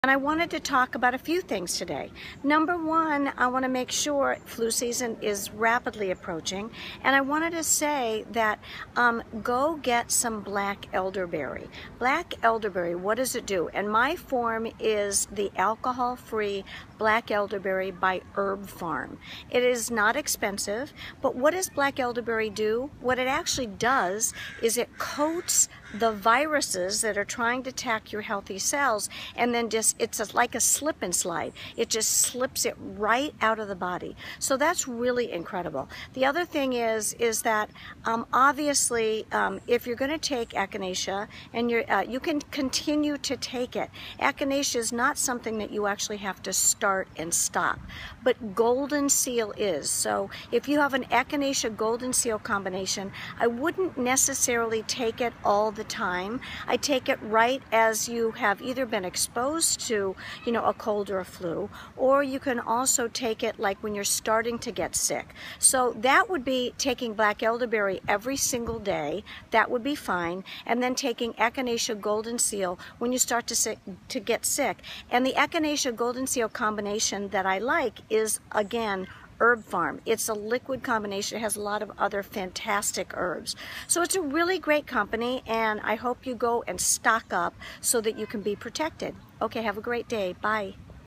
Protection against the flu
A few tips to protect yourself when you have been exposed to a cold or flu. More info or book a free, 15 minute phone consult with me by visiting